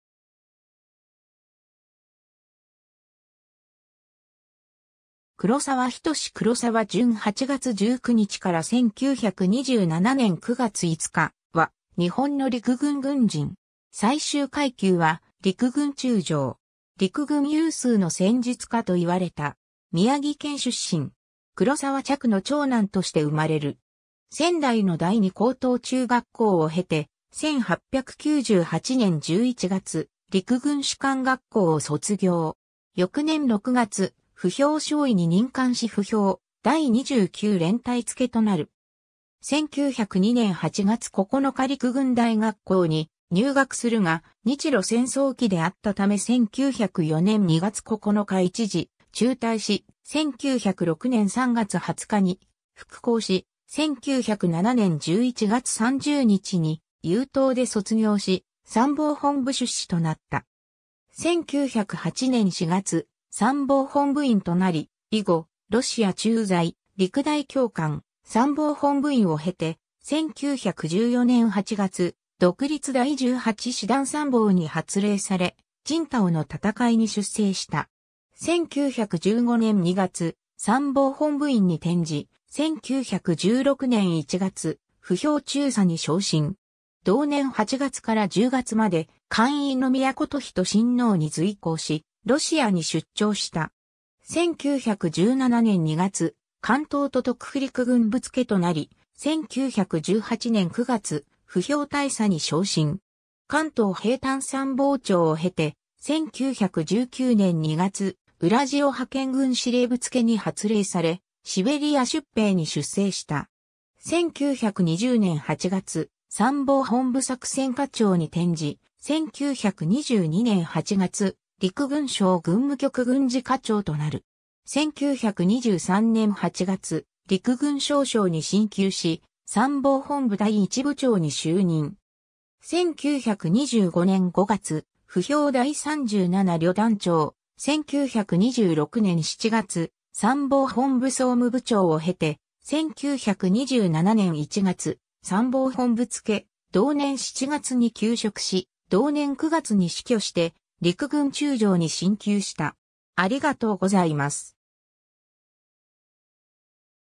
黒沢準
黒沢準 黒沢 準（黒澤、くろさわ ひとし、1878年（明治11年）8月19日 - 1927年（昭和2年）9月5日）は、日本の陸軍軍人。
陸軍有数の戦術家といわれた。
 宮城県出身。
黒沢嫡の長男として生れる。
仙台の第二高等中学校（後の旧制第二高校、その後東北帝国大学を経て現・東北大学）を経て、1898年（明治31年）11月、陸軍士官学校（10期）を卒業。
翌年6月、歩兵少尉に任官し歩兵第29連隊付となる。
1902年（明治35年）8月9日陸軍大学校（19期）に入学するが、日露戦争期であったため1904年（明治37年）2月9日一時中退し、1906年（明治39年）3月20日に復校し、1907年（明治40年）11月30日に優等で卒業し参謀本部出仕となった。
 1908年（明治41年）4月、参謀本部員となり、以後、ロシア駐在、陸大教官、参謀本部員を経て、1914年（大正3年）8月、独立第18師団参謀に発令され青島の戦いに出征した。
1915年（大正4年）2月、参謀本部員に転じ、1916年（大正5年）1月、歩兵中佐に昇進。
同年8月から10月まで閑院宮載仁親王に随行しロシアに出張した。
 1917年（大正6年）2月、関東都督府陸軍部付（ハルビン駐在）となり、1918年（大正7年）9月、歩兵大佐に昇進。
関東兵站参謀長（諜報担当）を経て、1919年（大正8年）2月、浦塩派遣軍司令部付（チタ特務機関長）に発令されシベリア出兵に出征した。
1920年（大正9年）8月、参謀本部作戦課長に転じ、1922年（大正11年）8月、陸軍省軍務局軍事課長となる。
1923年（大正12年）8月、陸軍少将に進級し参謀本部第1部長に就任。
1925年（大正14年）5月、歩兵第37旅団長、1926年（大正15年）7月、参謀本部総務部長を経て、1927年（昭和2年）1月、参謀本部付、同年7月に休職し、同年9月に死去して陸軍中将に進級した。